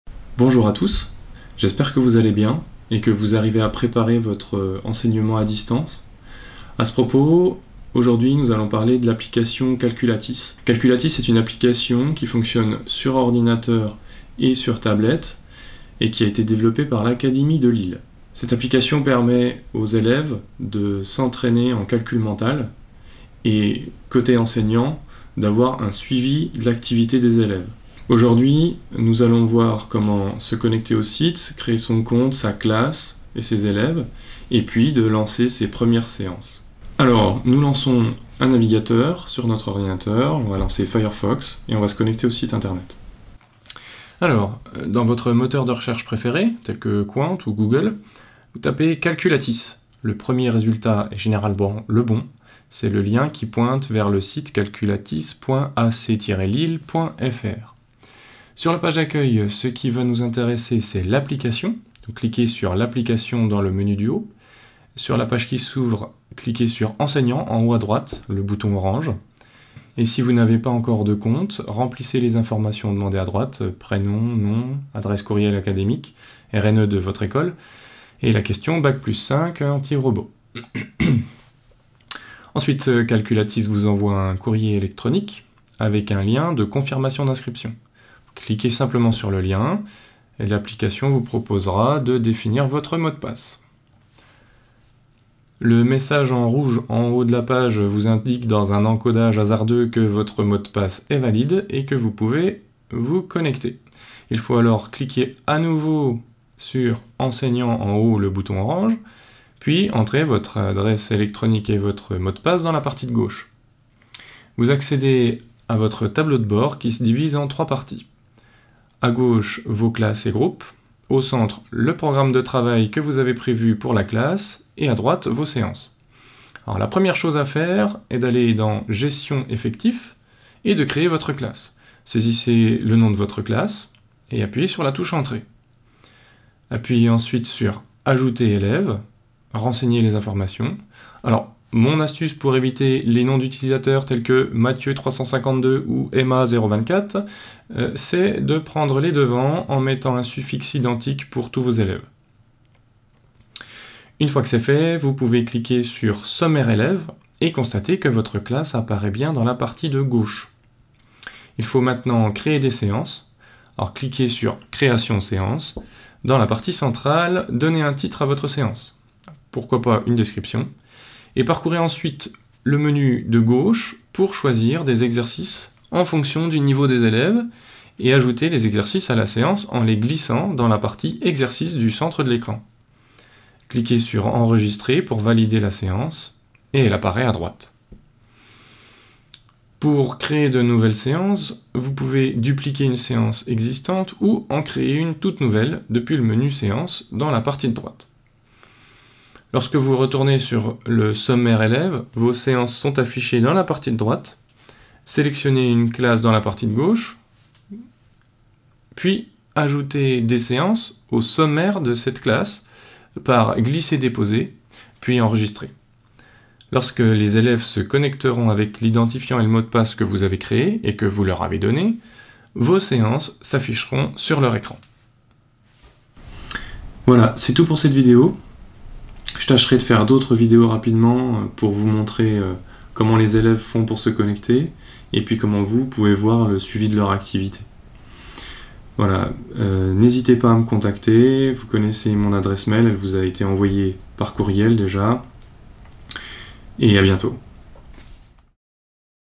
Calcul mental avec Calulatice - Créer ses premières séances
Calculatice est une plate-forme d'exercice au calcul mental avec suivi de l'activité des élèves. Elle propose des exercices d'entrainement du CP à la 6ème. Voici les étapes à suivre de la création de votre compte enseignant au lancement de votre première séance pour les élèves.